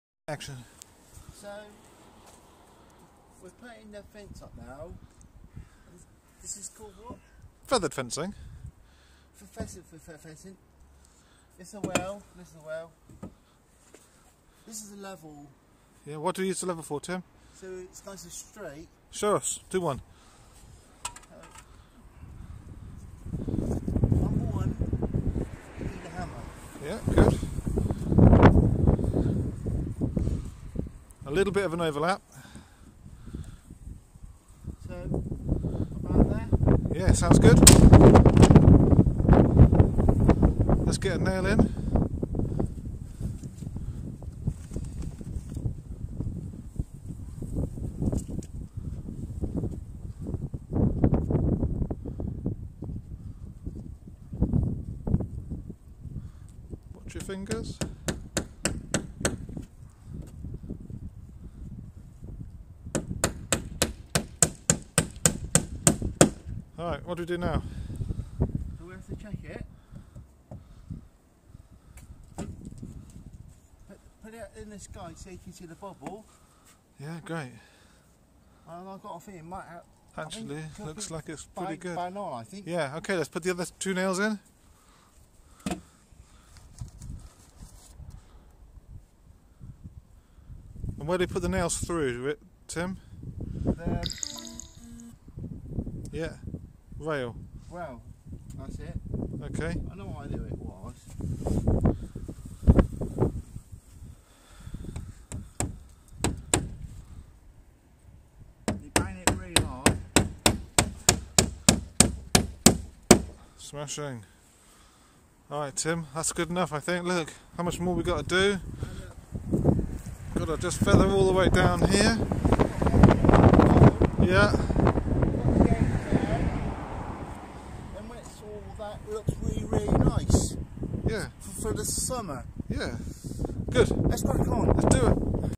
Tim fencing
How to put up feathered fence